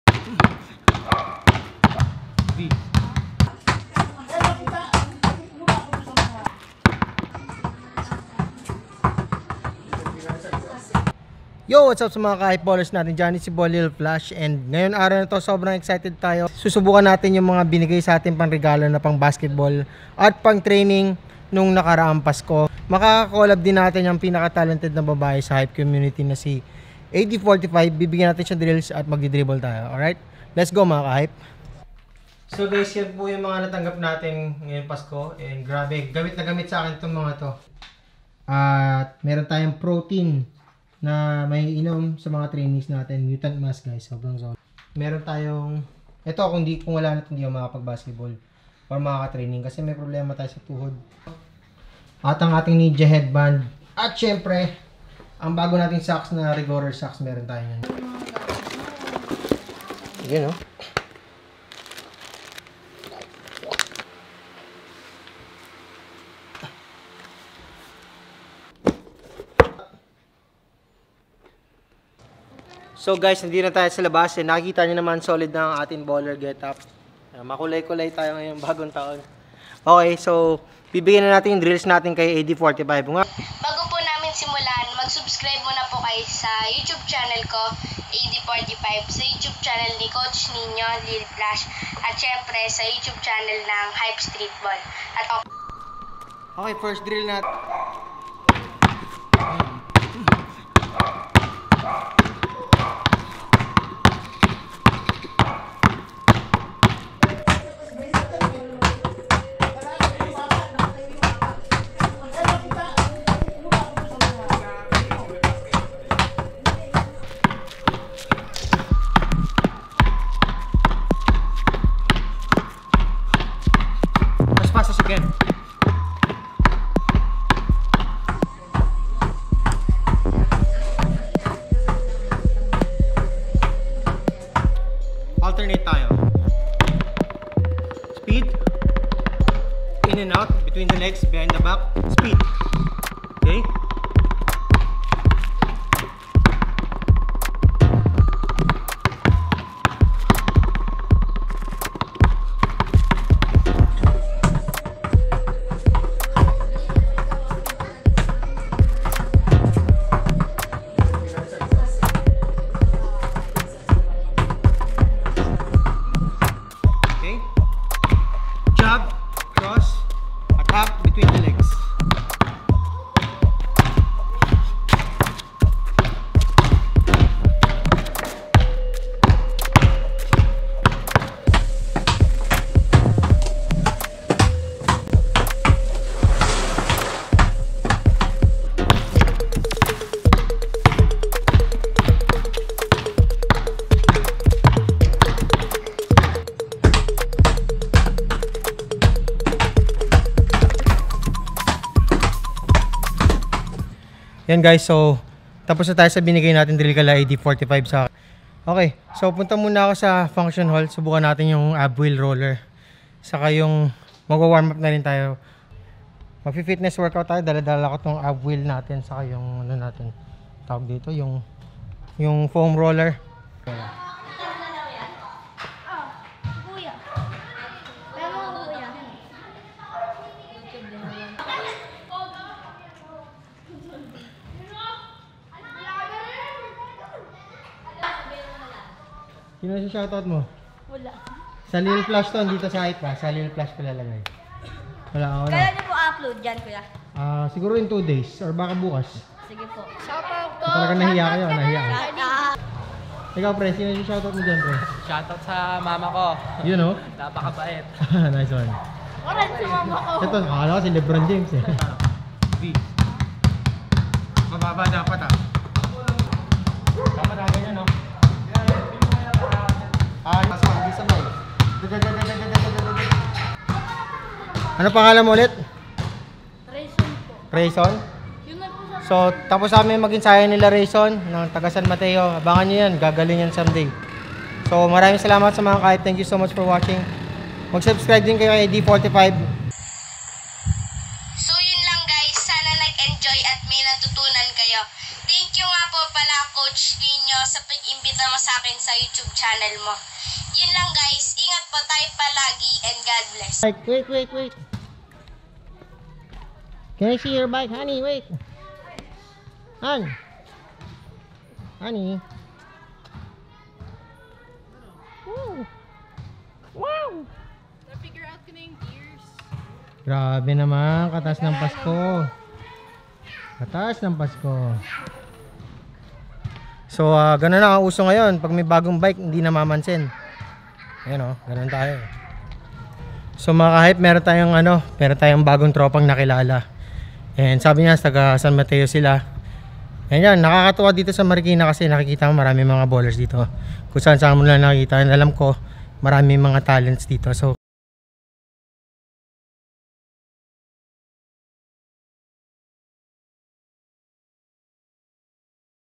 Ensayo Pang Bagong Taon - AD45 & Lil Flash
What's up mga Ka Hype nag online ensayo kame ni AD45 at may mga batang naki sali nalang samin ng baglaan at nagkaron kame ng group session ngayon bagong taon.